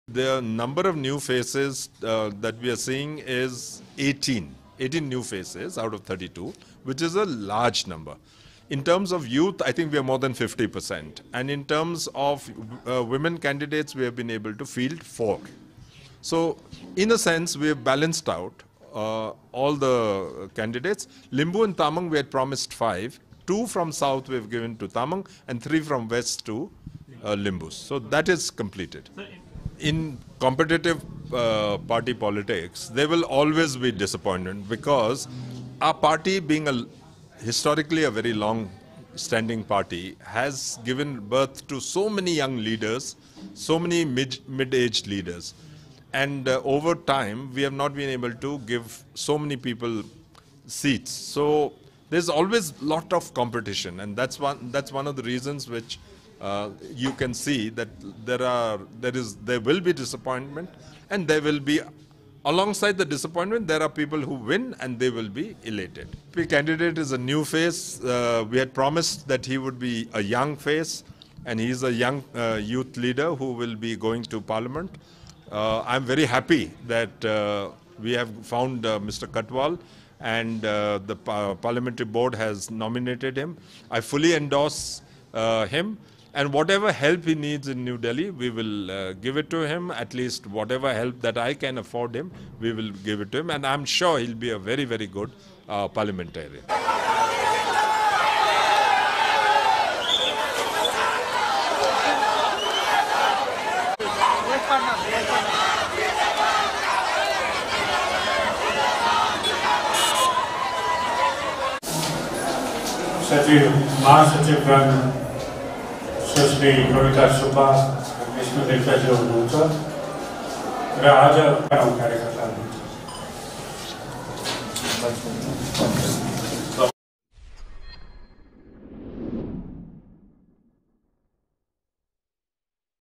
SDF announced final candidates list, inducted 18 new faces including Lok Sabha
Gangtok, March 22:

Sikkim Democratic Front party announced it's second and final list today. SDF has already listed out its 18 candidate list last week and today it announced 15 candidates including one lok sabha seats. There are 32 Assembly seats and One Lok Sabha seats.

After the declaration of candidates, SDF Party supporters vandalised the SDF Party HQ office situated in Indira byepass in Gangtok over final candidates declared today showing thier dissatisfaction.

Dozens of supporters ransacked party office, broke glass windows and furniture, while cops posted at the office tried to pacify and control the mob.

SDF has brought 18 new faces fulfilling the 50 percent youth and 50 old faces promised by CM Pawan Chamling. SDF has given new four women candidates.

This SDF has also inducted new face for Lok Sabha MP and DB Katwal will replace former MP PD Rai.

While the SDF has now declared final list but SKM has declared only 19 candidates